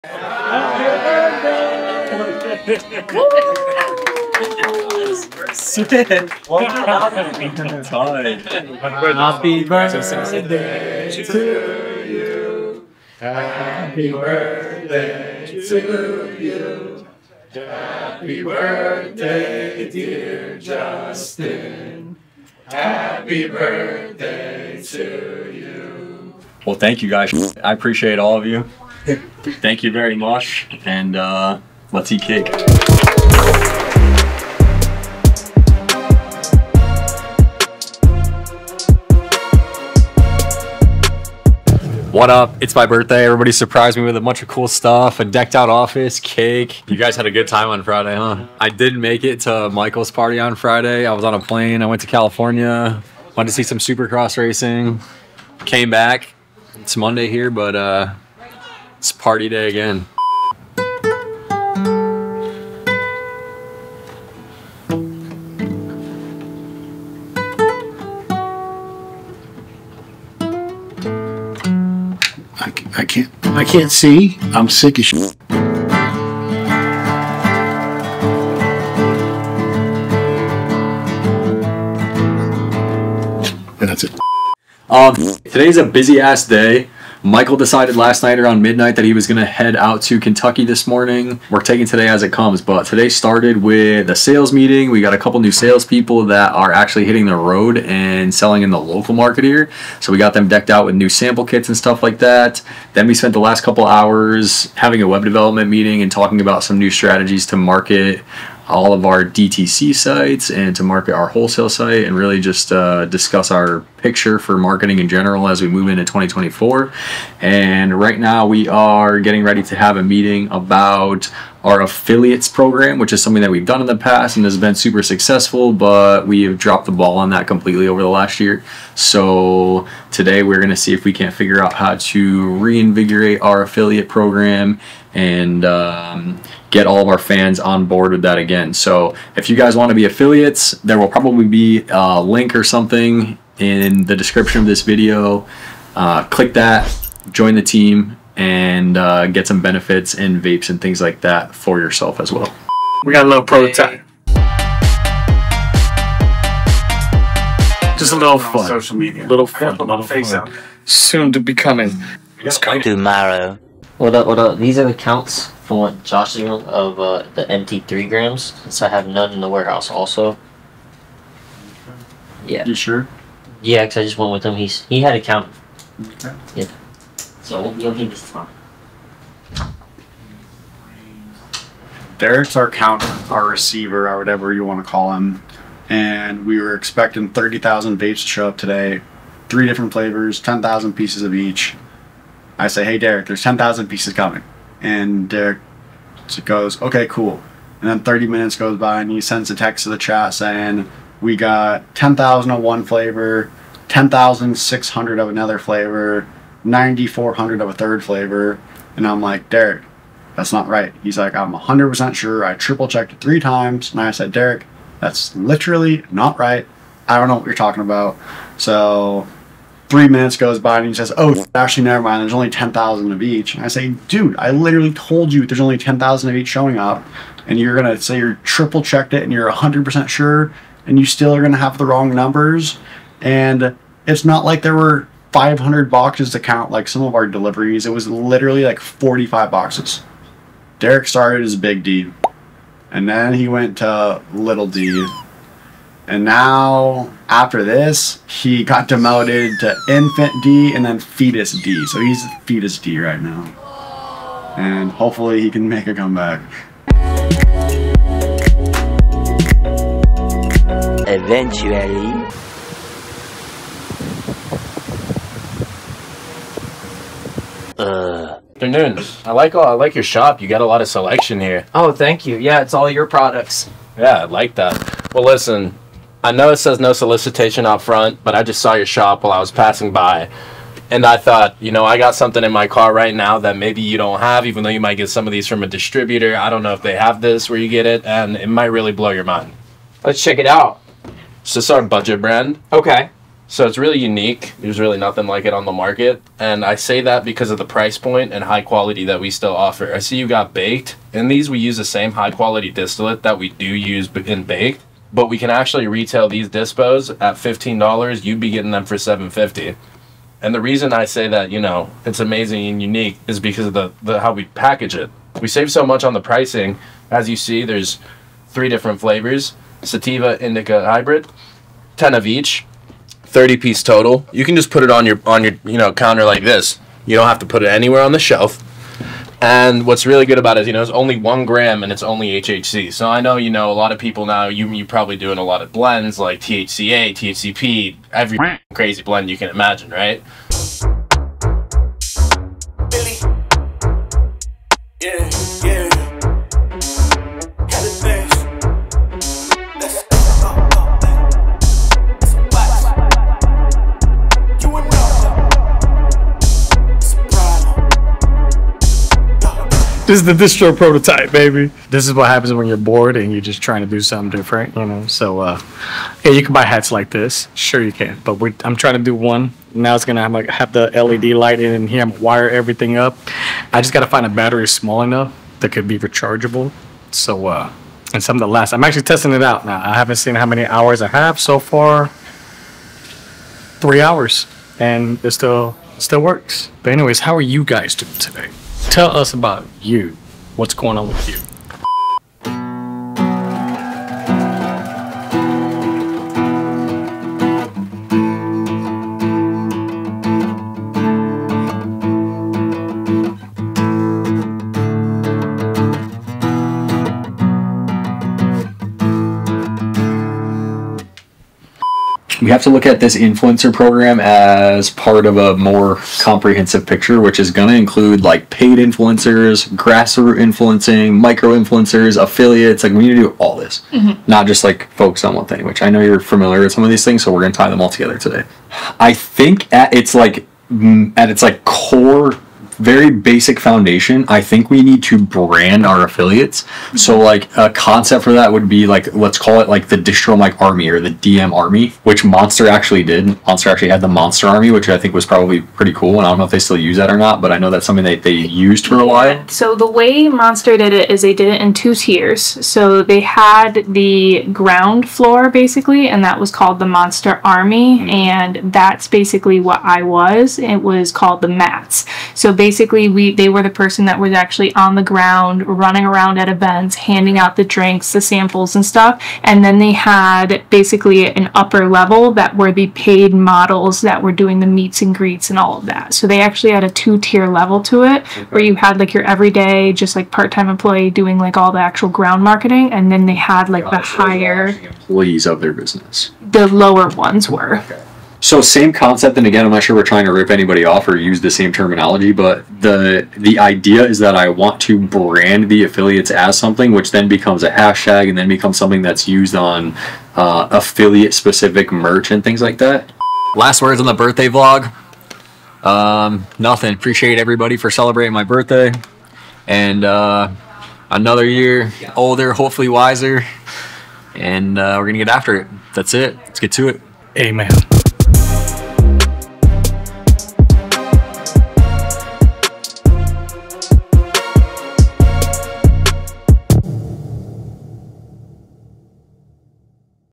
Justin's Distro Birthday - Distromike Vlog S1 E29
It's Justin's birthday! we planned a surprise for the birthday boy, Join us in this new Vlog.

🛒 Shop our Products: DistroMike.com

❤️ Thank you for your support! Your feedback fuels our creativity. Until next time, happy vaping! 🌬️🎉